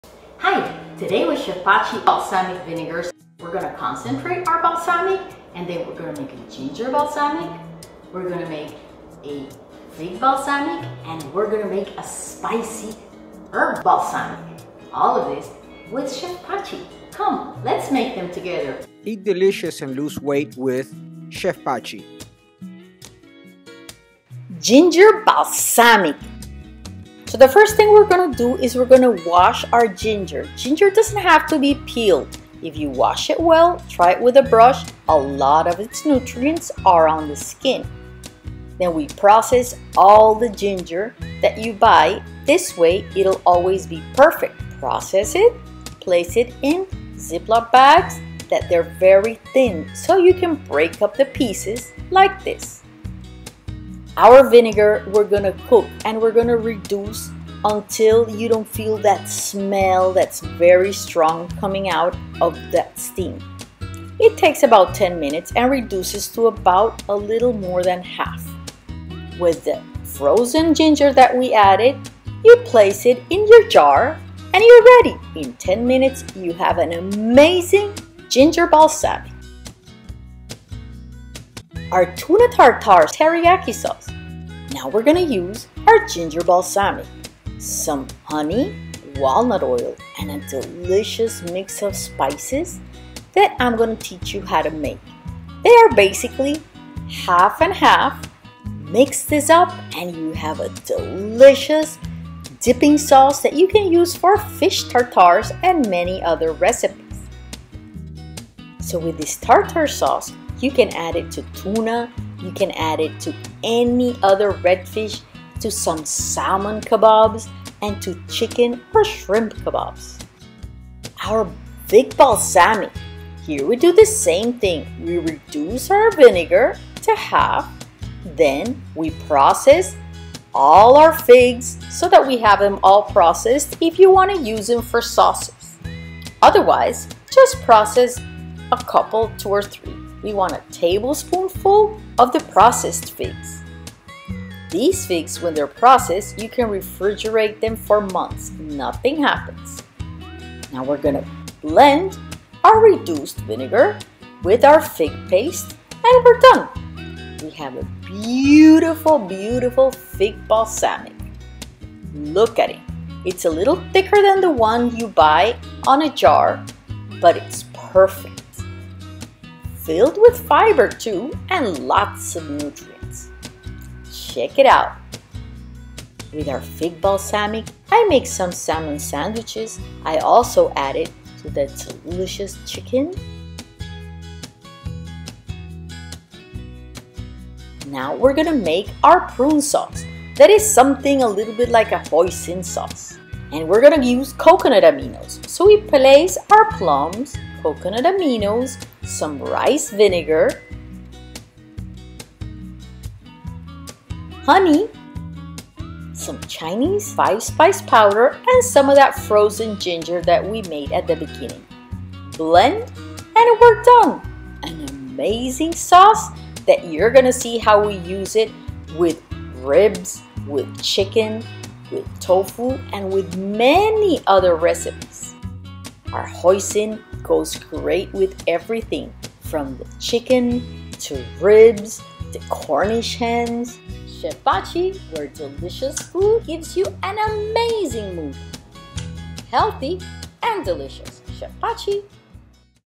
FOUR AMAZING SAUCES: with BALSAMIC VINEGAR: Ginger, Tartare, Fig & HOISIN type Prune and DUCK SAUCE
Wish you could flavor and season your meals with the maximum umami taste and naturally? No weird names, all ingredients you know? These amazing flavored balsamic and a duck sauce: Ginger, Tuna Tartare Sauce, Fig Balsamic, along with a HOISIN type Prune and DUCK SAUCE will enlighten your healthy meals, make your family love the food, and get used to a lifestyle of healthy and delicious diet plan. No one will believe the food is healthy and easy to make too. You can also use them to plate and dot your dishes as restaurants do. They will serve you all your new and traditional dishes. I would love to know, write me and tell me!

3 Fig Balsamic
1 cup of balsamic vinegar
3 figs or 1 tablespoon ground figs

Gingered balsamic
1 cup balsamic
2 tablespoons grated ginger

1. Place balsamic in a small pot and bring to a boil. Simmer for 8 minutes. Remove from the heat, add ginger and keep in a covered bottle or jar.

Tuna and Salmon Tartare Sauce
2 tablespoons gingered balsamic
1 tablespoon honey
1 tablespoon walnut oil
1/4 teaspoon Chef Pachi Seafood Spice Mix or Old Bay

Chef Pachi’s Plum Hoisin Style Sauce (GF-Low Salt)
10-12 pitted dried plums
3 tablespoons Coconut aminos
2 tablespoons grated ginger
2 tablespoons honey
2 tablespoon Rice vinegar
1/2 teaspoon Chinese 5 Spice Powder